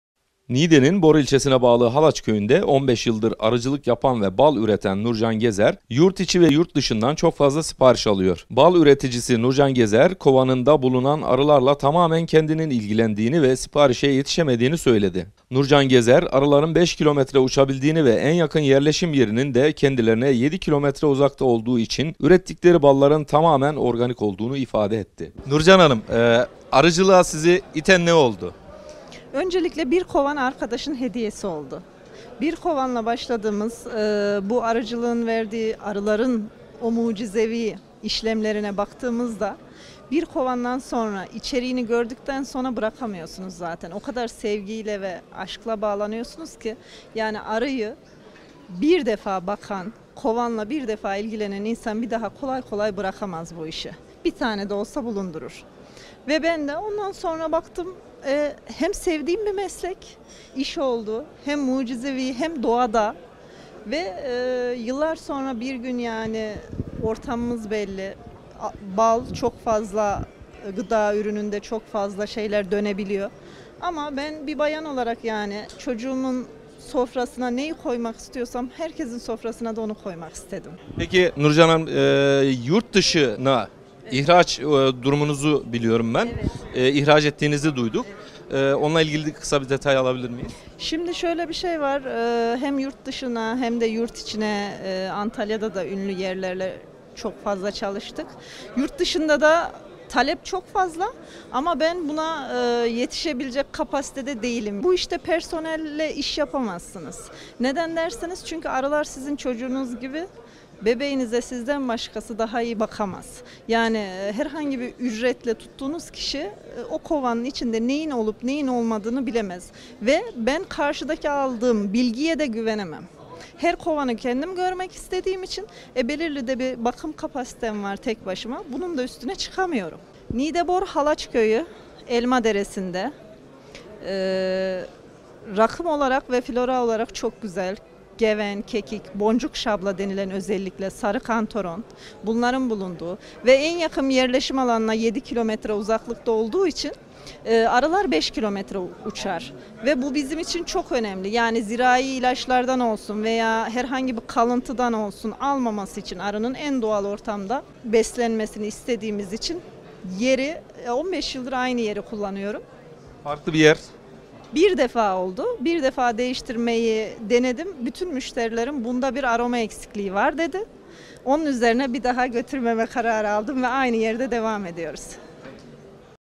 Arkadaşının hediyesiyle başladı, şimdi siparişlere yetişemiyor
Niğde’nin Bor İlçesine bağlı Halaç köyünde 15 yıldır arıcılık yapan ve bal üreten Nurcan Gezer, yurt içi ve yurt dışından çok fazla sipariş alıyor.
Arkadaşının hediye ettiği bir kovanla bal üretmeye başlayan Nurcan Gezer, kovanında bulunan arılarla tamamen kendisinin ilgilendiğini ve siparişlere yetişemediğini söyledi.
Nurcan Gezer, arıların 5 kilometre uçabildiğini ve en yakın yerleşim yerinin de kendilerine 7 kilometre uzakta olduğu için, ürettikleri balların tamamen organik olduğunu ifade etti.